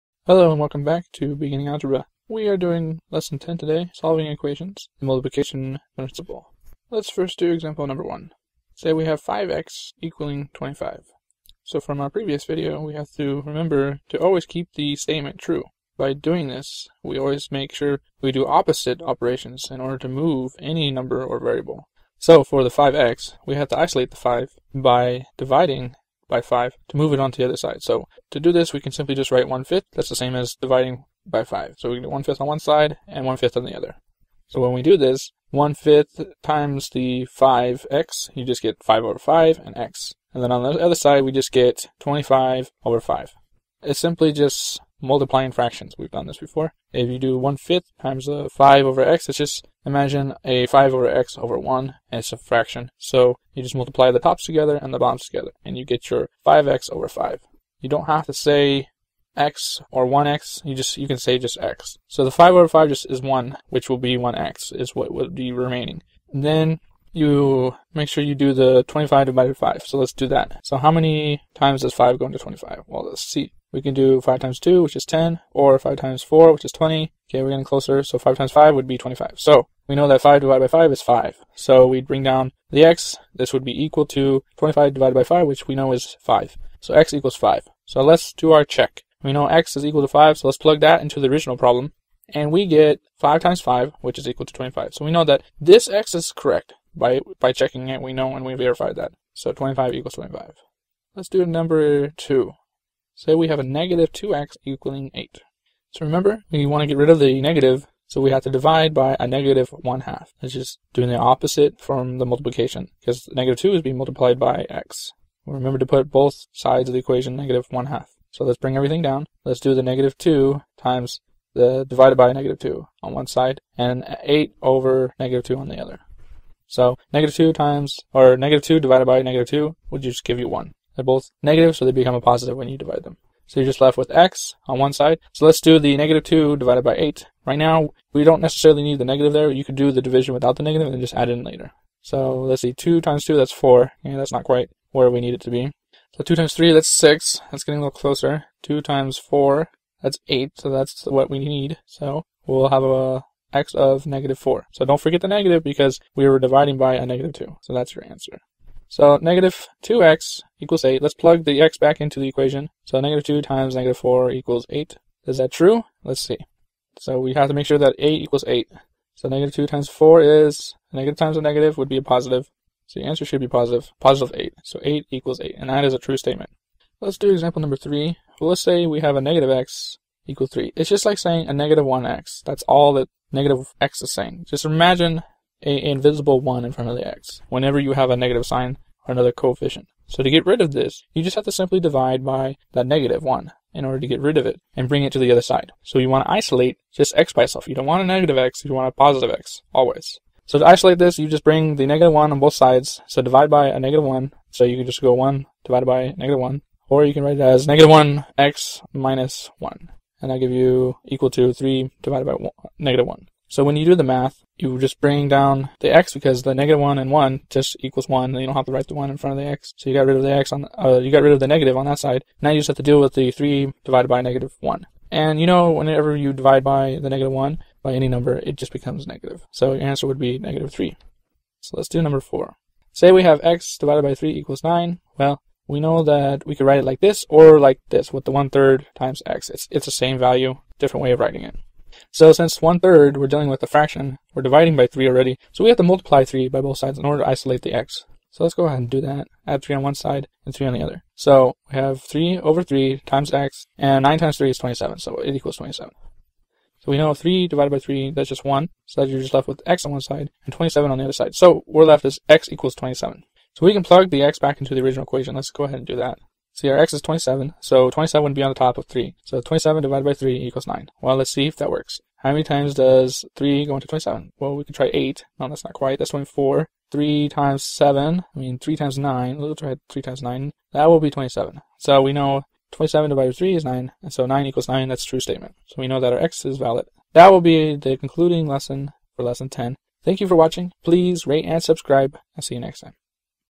Solving Equations-The Multiplication Principle: Beginning Algebra (L10)
Solving equations for x values using multiplication and division method.